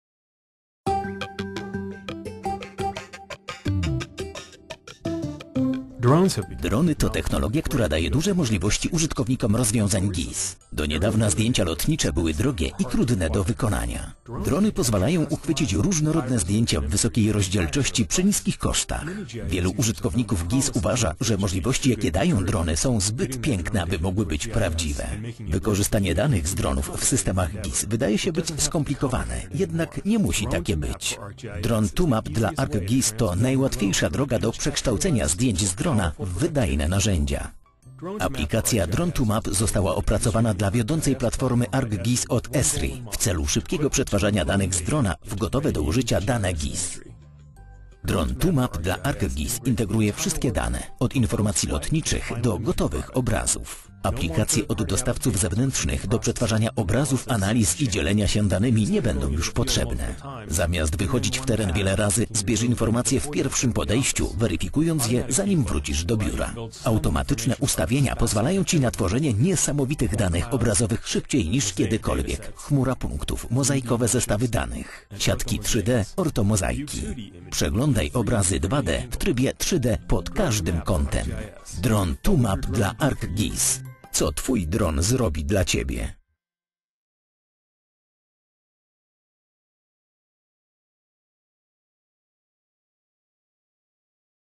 Drone2Map for ArcGIS
Drony mogą być wykorzystywane do monitorowania środowiska, zarządzania kryzysowego, analizy terenu, kontroli infrastruktury i wielu innych. Podczas Esri UC wprowadzono nowy produkt Drone2Map for ArcGIS - aplikację desktopową, która w ciągu kilku minut zamienia obrazy z dronów na profesjonalne produkty 2D i 3D, takie jak mapy internetowe w ArcGIS. Przetwarzanie takich obrazów tradycyjną metodą zajęłoby wiele dni. Drone2Map jest częścią ArcGIS Online, dzięki czemu użytkownicy mogą od razu z nich korzystać.